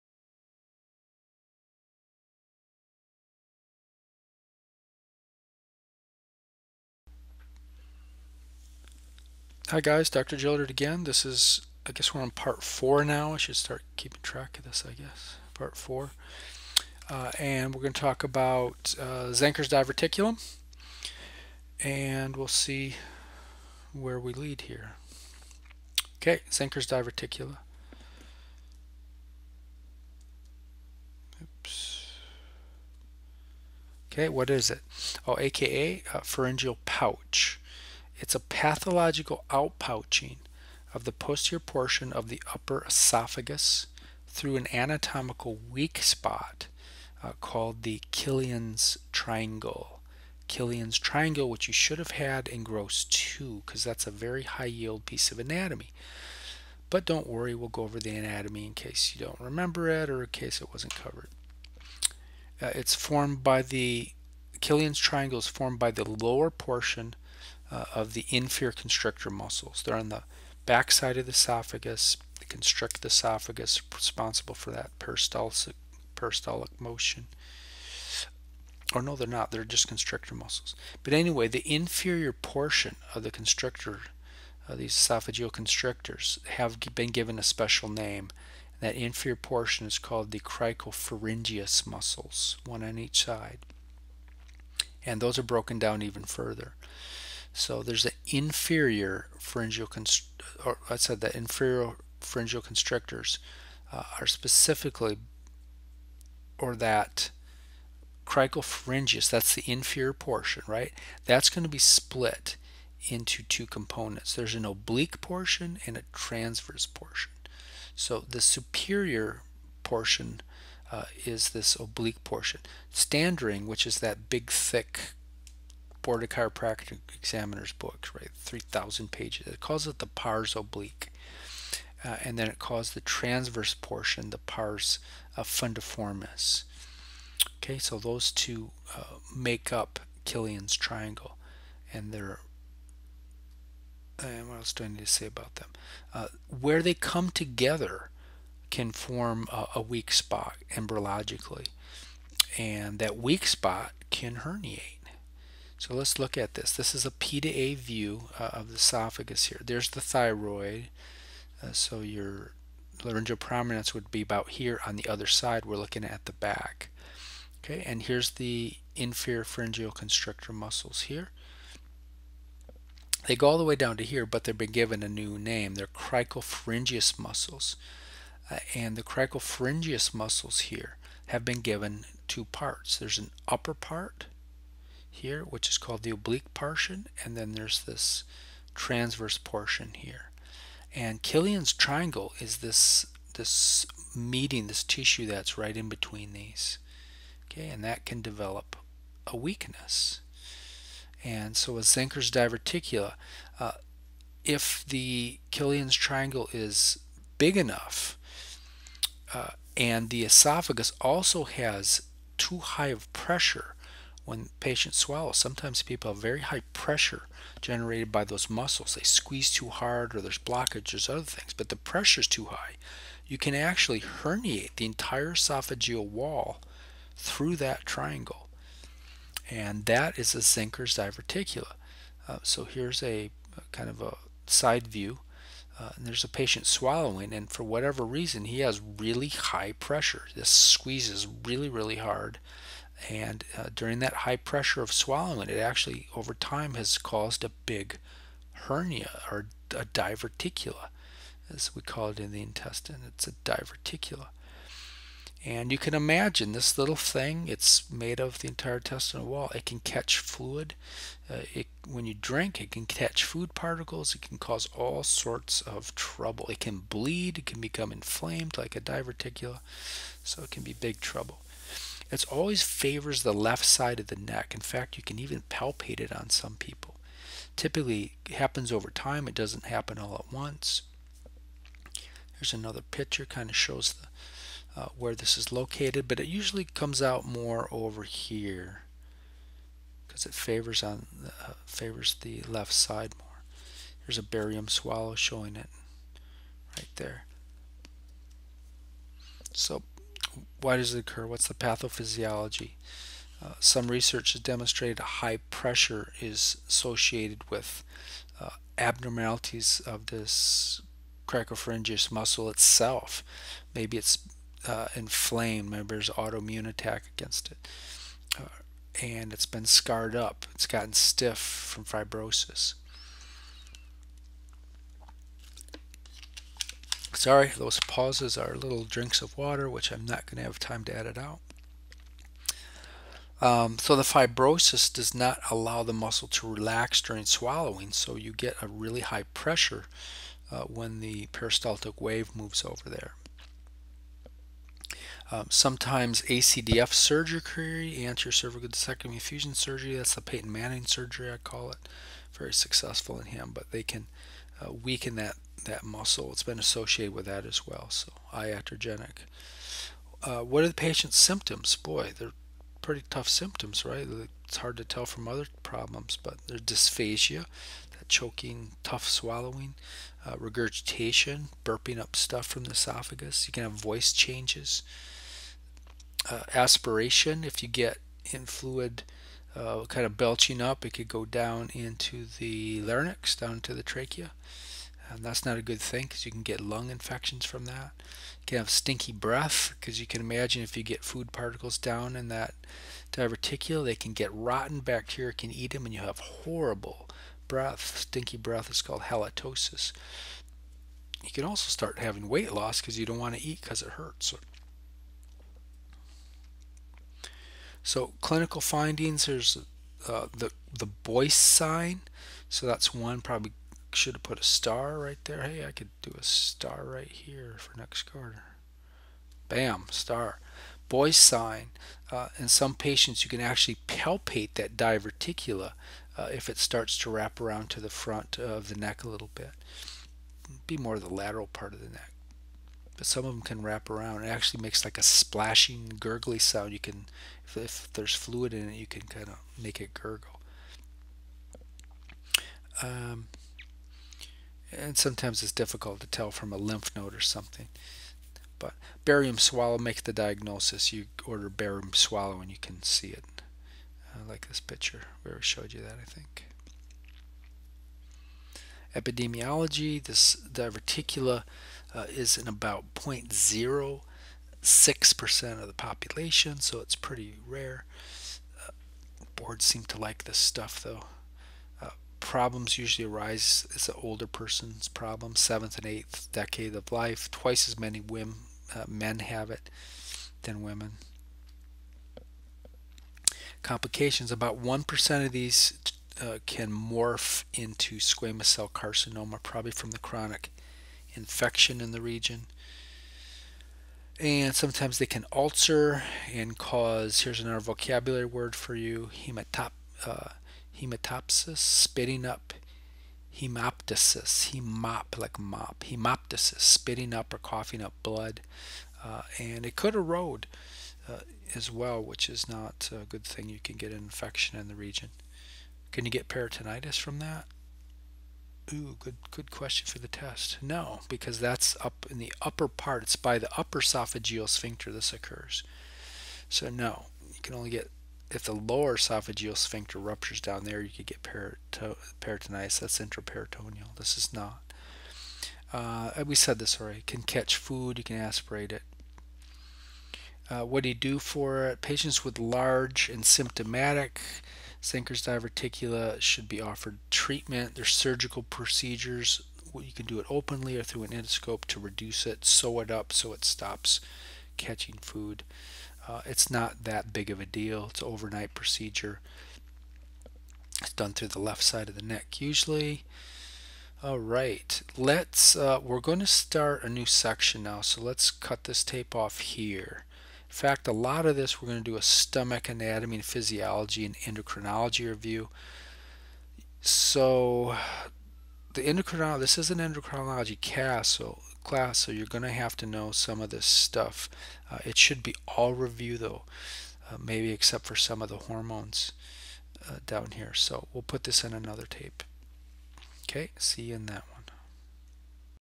Zenker's diverticula Part 4 of the makeup lecture.
In this lecture, Dr. Gillard talks about Zenker's diverticula.  This is part 4 of the makeup lecture for 1/18/18.